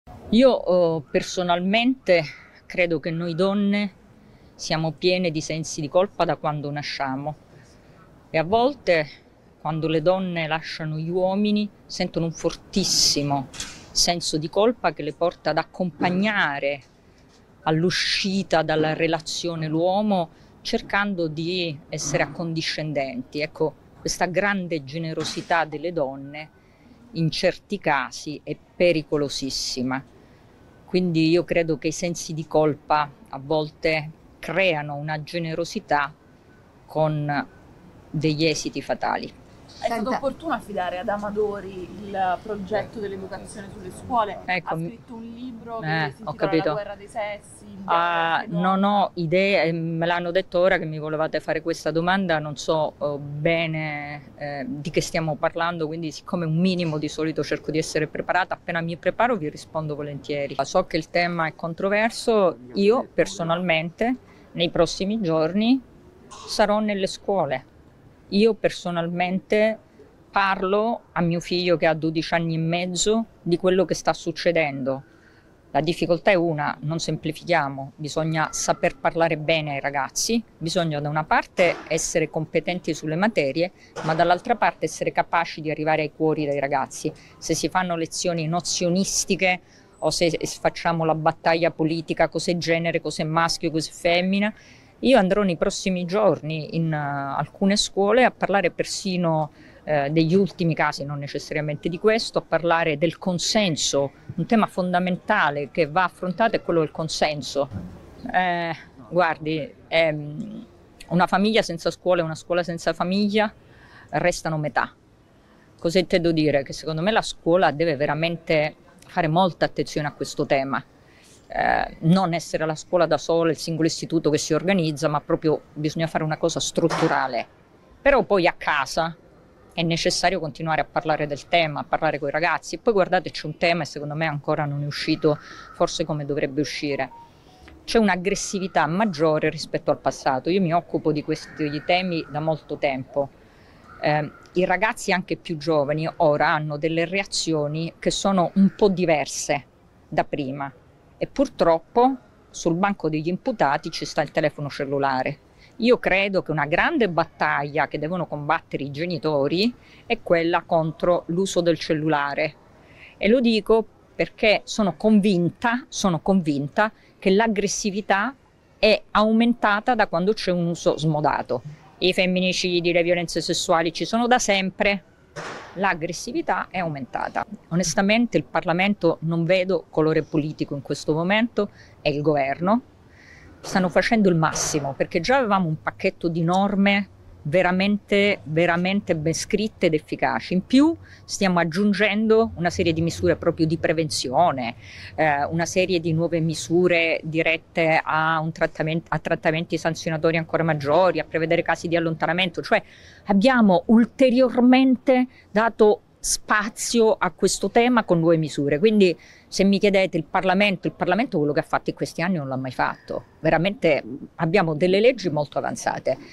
Violenza di genere, Bongiorno: "Governo fa il massimo. In Italia abbiamo leggi avanzate"
"Amadori? Mi devo informare prima di parlarne"

POLITICA (Roma). "Io penso che noi donne siamo piene di sensi di colpa fin da quando nasciamo. A volte quando lasciamo un uomo diventiamo troppo generose e questa cosa è pericolosa e fatale". Così la senatrice della Lega, Giulia Bongiorno, nel corso di un punto stampa tenutosi a San Luigi dei francesi. "Amadori? Mi devo informare prima di parlarne", ha continuato. Poi ha proseguito definendo l'educazione affettiva è "un tema controverso, io personalmente sarò nelle scuole. Bisogna saper parlare bene ai ragazzi per arrivare ai loro cuori. Una famiglia senza scuola e viceversa, restano metà. La scuola deve fare molta attenzione a questo tema, ma è necessario parlarne anche a casa. I cellulari sono pericolosi perché hanno portato all'aumento dell'aggressività". Mentre sul cosa può fare la politica ha detto: "il governo sta facendo il massimo che sta aggiungendo misure di prevenzione e dà ulteriormente spazio a nuove misure. Abbiamo leggi avanzate". (Davide Di Carlo/alanews)

Io personalmente credo che noi donne siamo piene di sensi di colpa da quando nasciamo e a volte quando le donne lasciano gli uomini sentono un fortissimo senso di colpa che le porta ad accompagnare all'uscita dalla relazione l'uomo cercando di essere accondiscendenti Ecco questa grande generosità delle donne in certi casi è pericolosissima quindi io credo che i sensi di colpa a volte creano una generosi con degli esiti fatali ho capito Non ho idea e me l'hanno detto ora che mi volevate fare questa domanda non so bene di che stiamo parlando quindi siccome un minimo di solito cerco di essere preparata Appena mi preparo vi  ma so che il tema e controverso io personalmente nei prossimi giorni sarò nelle scuole io personalmente parlo a mio figlio ha 12 anni e mezzo di quello che sta succedendo la difficoltà è una non semplifichiamo bisogna saper parlare bene ai ragazzi bisogno da una parte essere competenti sulle materie ma dall'altra parte essere capaci di arrivare ai cuori dai ragazzi se si fanno lezioni nozioni stick e hostess facciamo la battaglia politica cose genere così è maschio o femmina Io andrò nei prossimi giorni In alcune scuole a parlare persino degli ultimi casi non necessariamente di questo o parlare del consenso in tema fondamentale che va affrontato e quello il consenso guardi una famiglia senza scuola è una scuola senza famiglia restano metà  cosa intendo dire che secondo me la scuola deve veramente fare molta attenzione a questo tema non essere la scuola da solo è single Istituto che si organizza ma proprio bisogna fare una cosa strutturale però poi a casa è necessario continuare a parlare del tema parlare coi ragazzi e poi guardate C'è un tema e secondo me ancora non è uscito Forse come dovrebbe uscire cioè un'aggressività maggior rispetto al passato io mi occupo di questi di temi da molto tempo i ragazzi anche più giovani ora hanno delle reazioni che sono un po' diverse da prima purtroppo sul banco degli imputati ci sta il telefono cellulare Io credo che una grande battaglia che devono combattere i genitori e quella contro l'uso del cellulare e lo dico perché sono convinta Sono convinta che l'aggressività è aumentata Da quando c'è un uso smodato i femminicidi e violenze sessuali ci sono da sempre  l'aggressività è aumentata onestamente il Parlamento non vedo colore politico in questo momento è il governo stanno facendo il massimo perché già avevamo un pacchetto di norme veramente veramente belle scritte ed efficaci in più stiamo aggiungendo una serie di misura proprio di prevenzione una serie di nuove misure dirette a un trattamento sanzionatorio ancora maggiori a prevedere casi di allontanamento Cioè abbiamo ulteriormente dato spazio a questo tema con due misure Quindi se mi chiedete il Parlamento il Parlamento quello che ha fatto in questi anni non l'ha mai fatto veramente abbiamo delle leggi molto avanzate